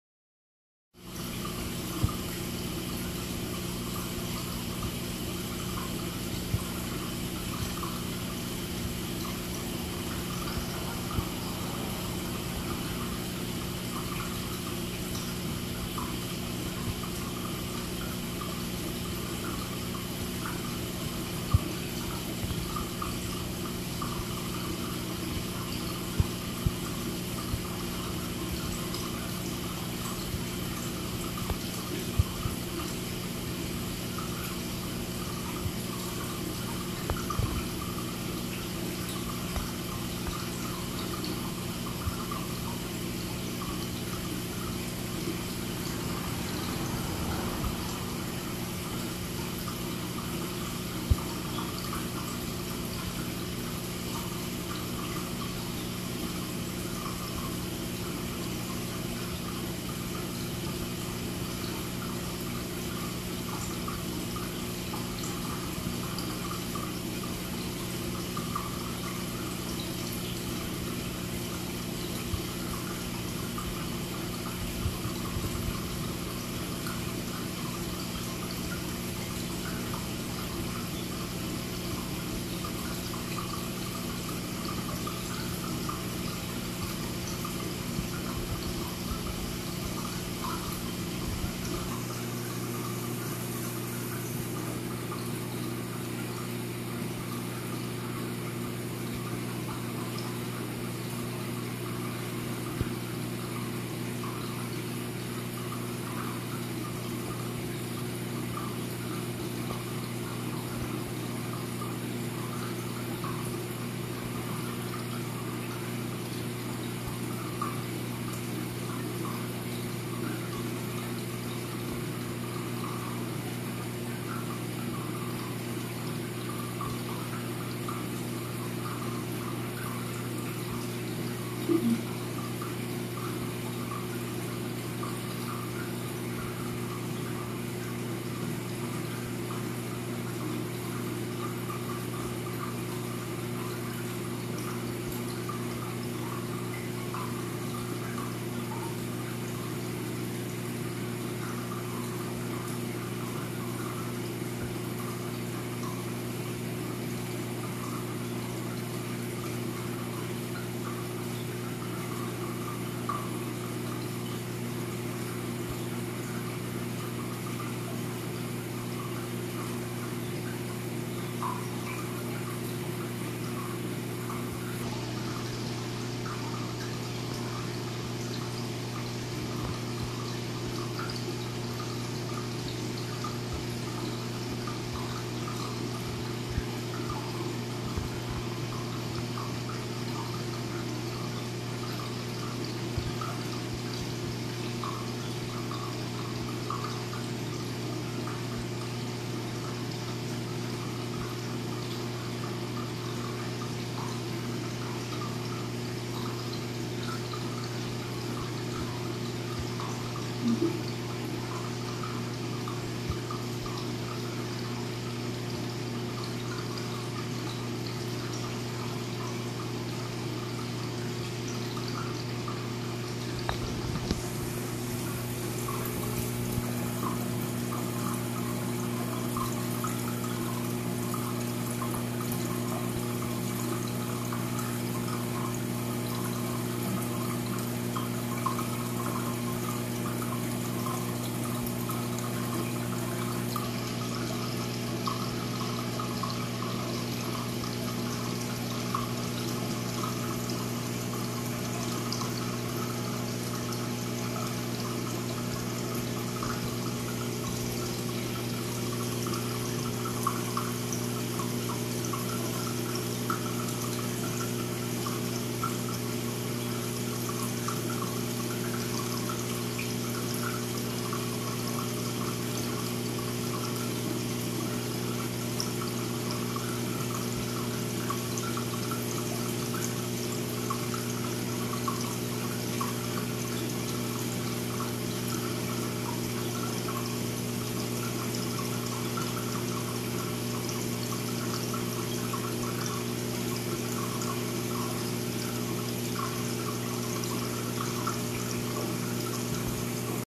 Messing with the CURRENT USA LED modes
Disregard the daylight blasting thru the gap in the blinds.. the dog decided I apparently didnt need that slat..